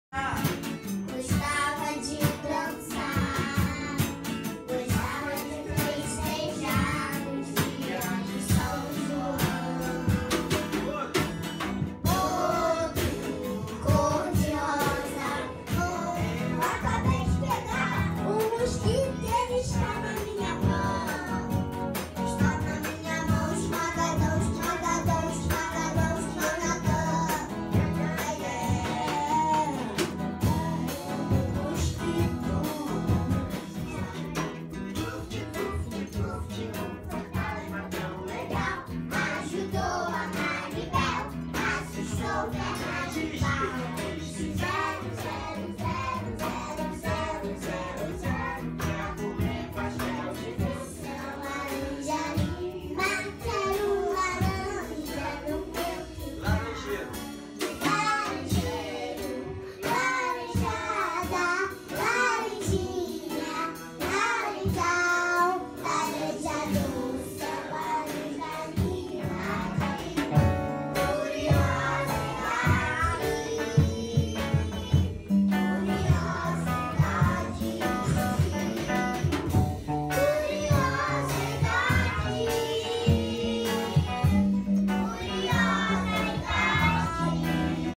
Ensaio do Grupo 5 Manhã para o "Sábado Cultural" do dia 24/09/2016
Os alunos do Grupo 5 Manhã foram ao Espaço 23, do coordenador José Henrique Nogueira, para ensaiar para o evento Sábado Cultural, que acontecerá no Teatro Cacilda Becker, em 24/09/2016.